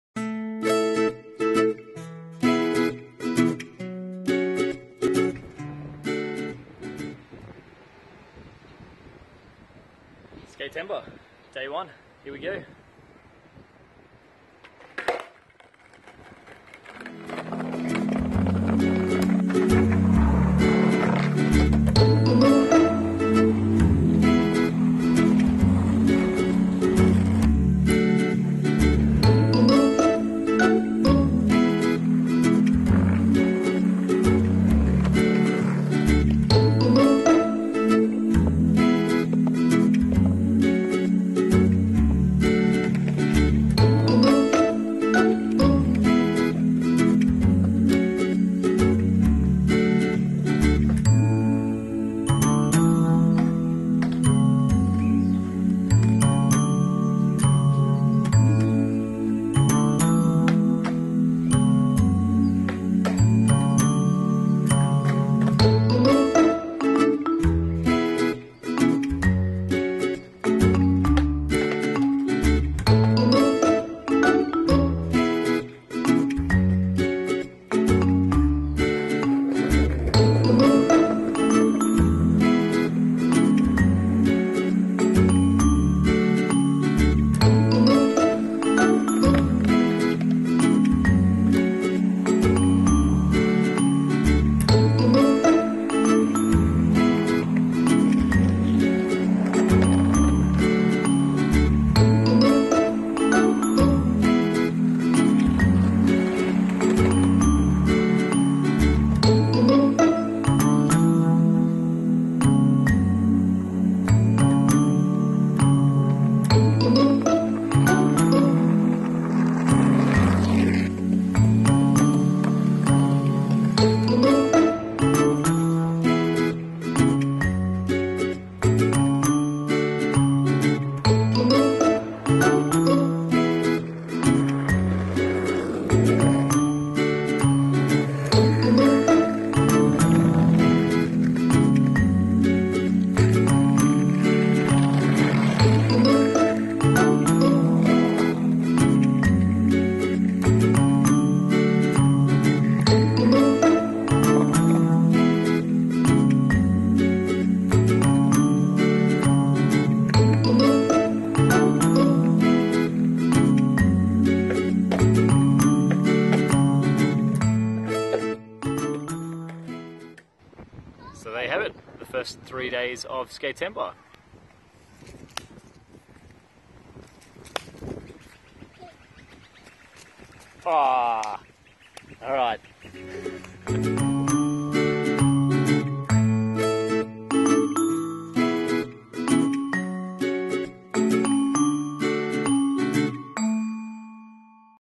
Skatember 1-3
A montage of my first 3 days of Skatember 2016. I had a lot of fun making this video and skating every day.